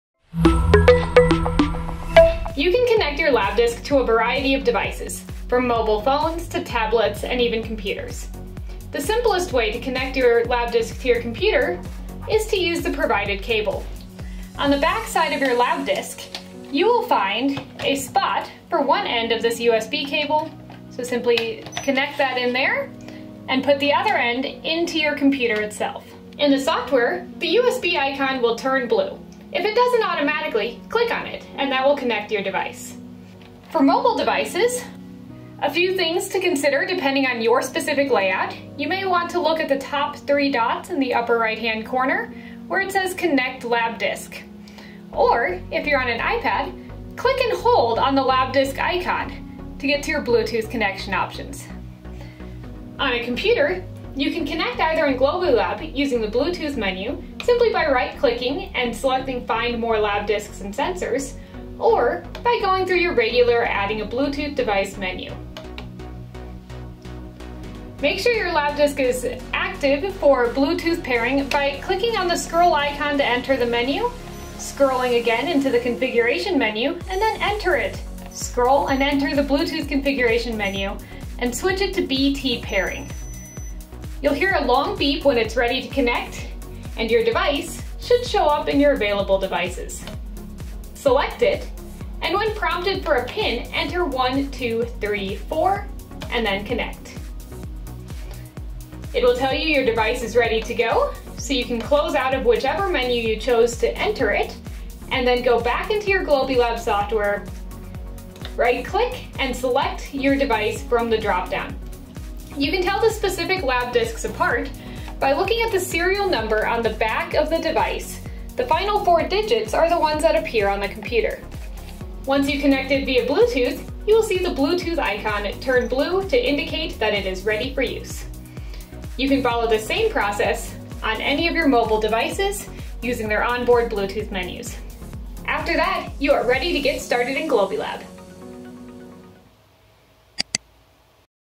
Connecting Your Labdisc to Your Device
The Labdisc Portable STEM Lab can be connected to almost any device. Find out how to go about doing that in this quick video.

You can connect your Labdisc using the provided cable to a USB port or via Bluetooth to any compatible device to connect to your Globilab software.

-Make sure your Labdisc Bluetooth is enabled.
(Enter Menu/Configuration/Bluetooth/BT Pairing)
-Connect your Labdisc like any other Bluetooth device.
-When prompted, use the pin 1234
-You can differentiate the individual Labdiscs using the Serial Numbers found on the reverse of the device. The final four digits correspond to the name of the Labdisc as it appears on your device. Labdisc_
-If it doesn't immediately connect, press and hold the scroll button until the bluetooth icon appears and a beep sounds. While the beep is sounding, press enter on your device after entering 1234. It's important that these happen simultaneously for it to connect successfully.

This video is part of our new online training course on the Labdisc, which you can access